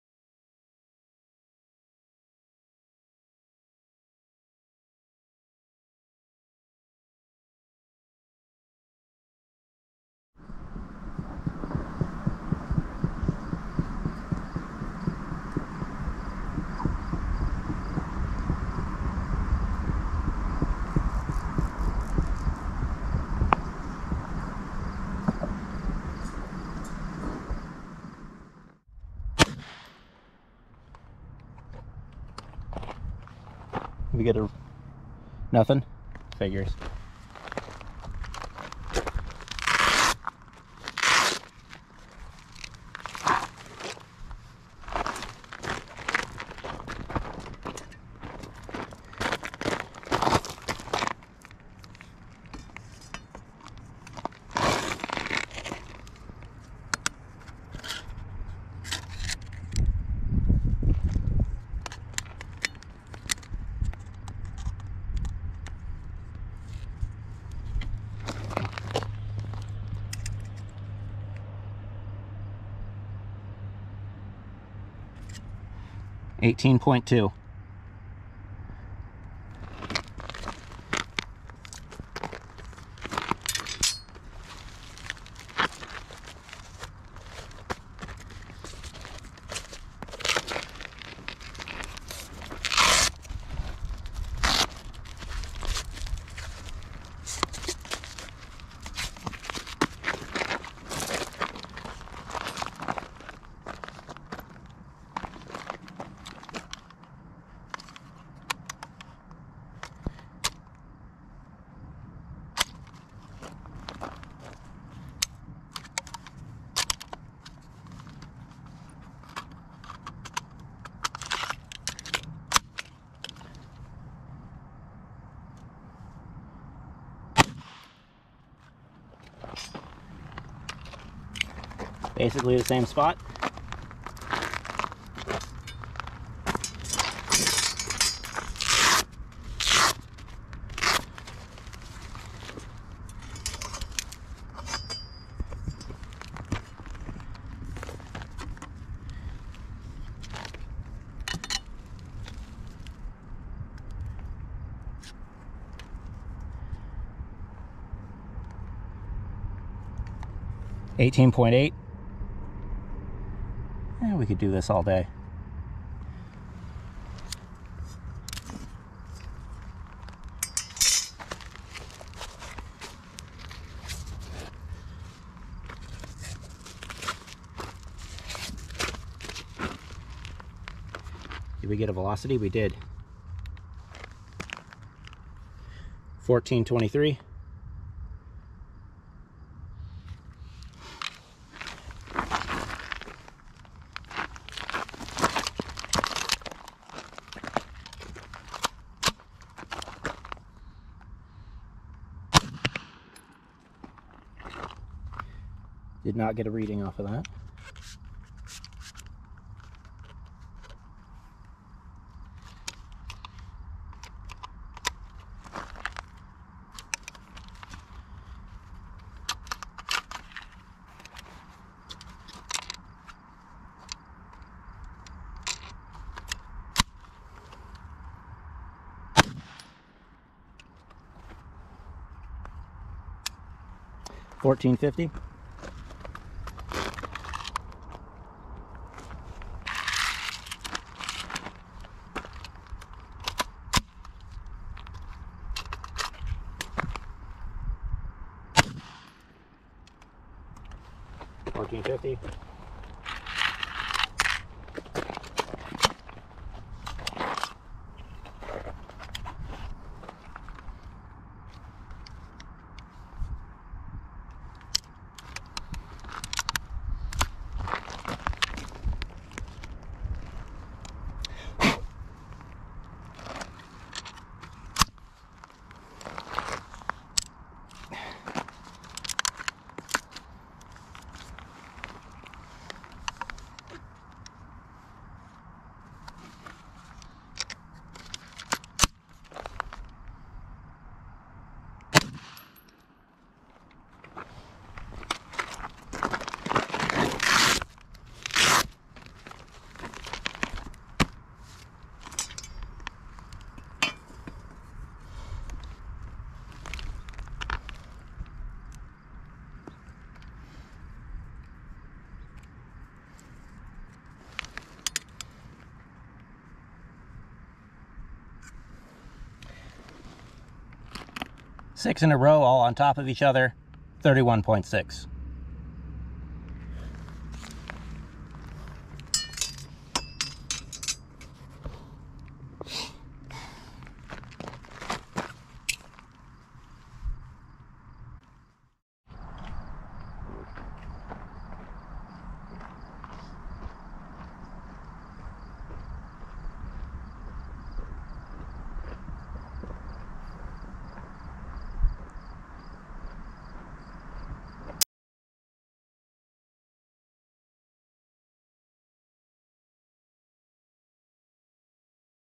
Red Star Defense 3A Vest Fall 2021 Batch Test
Test of the newest stock of level 3a ballistic vests.

Passing is defined as:
1. Zero pentation
2. Backface deformation of less than 44mm, as is the standard for all NIJ levels

Tested with:
6 rounds of 124gr 9mm FMJ +P+ at ~1,440fps
Less than 3ft distance from target
Less than 1.5" grouping

This test is not meant to be an approximation of an NIJ level 3A test. These vests have been lab tested previously, and we just want to show how they perform under multiple hits from a very common cartridge, loaded very hot, in a very tight grouping. We also use this data to benchmark against similar previous tests.